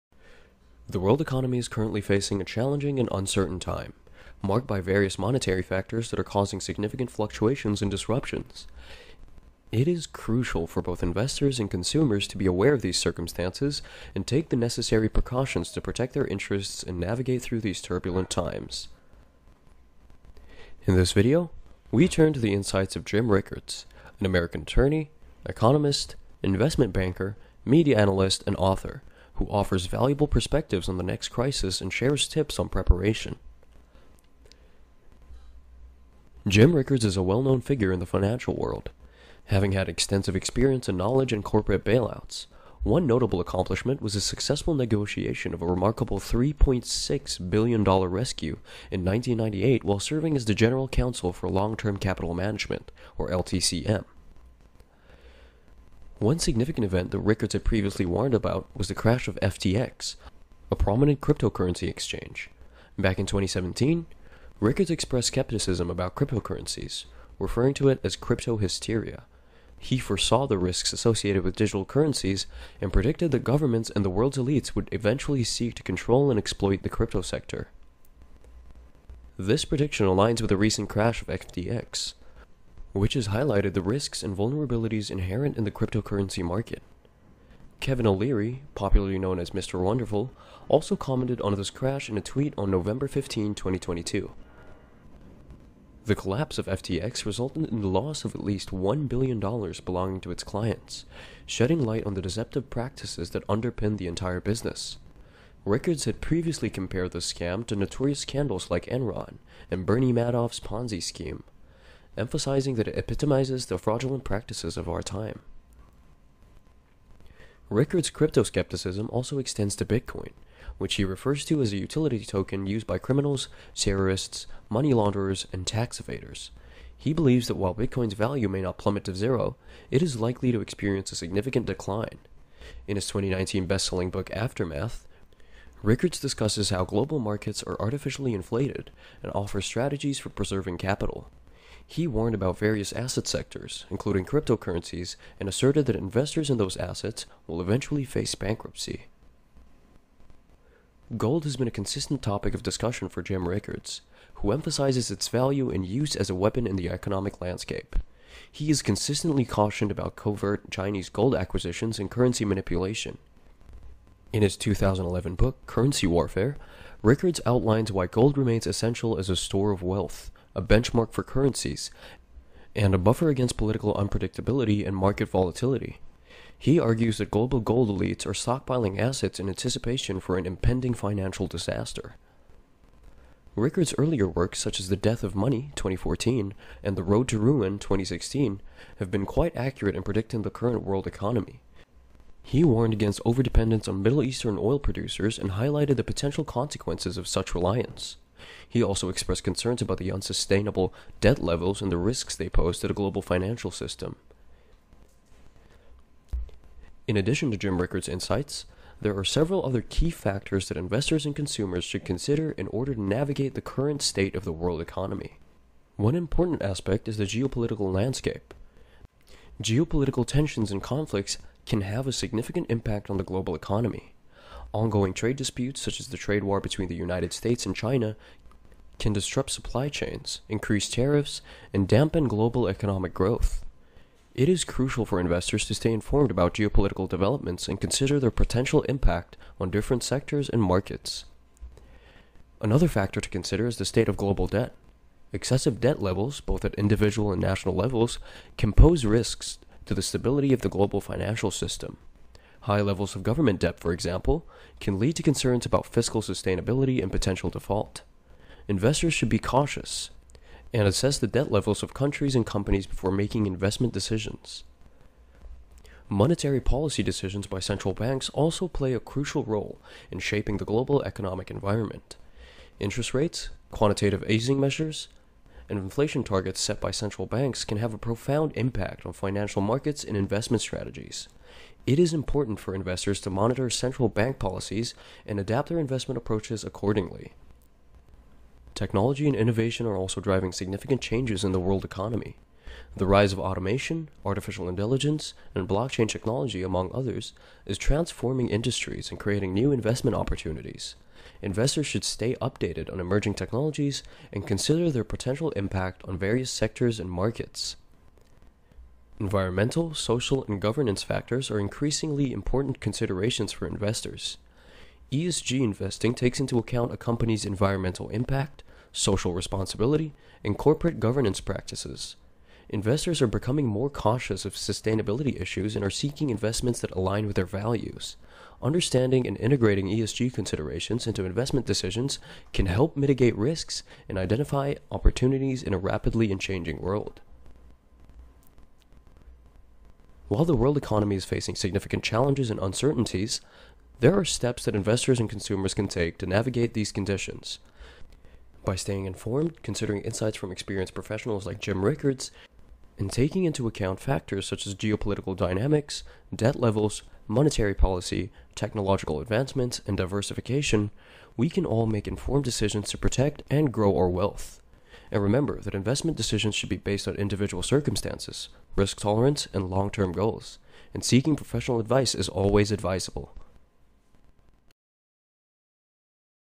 Invest Your 10% Cash in this Asset Now Fed Don't Want You To Know This Jim Rickards
In this video, renowned financial expert Jim Rickards reveals a groundbreaking investment opportunity that the Federal Reserve doesn't want you to know about. By investing just 10% of your cash in this asset, you can potentially safeguard your wealth and maximize your returns. Gain access to this exclusive information that could help you navigate the challenges of the impending recession in 2023. Don't miss out on this free investing advice from Jim Rickards! Tune in now to learn more and secure your financial future.